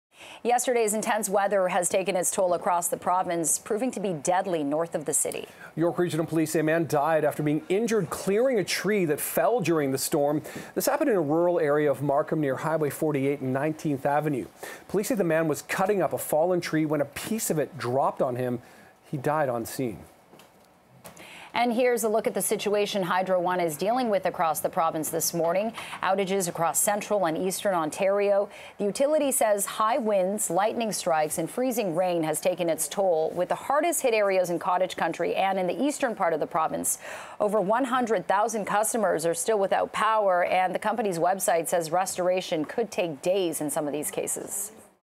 Man killed while cutting up a tree in Markham
Yesterday's intese weather has taken its toll across the province, proving deadly north of the city.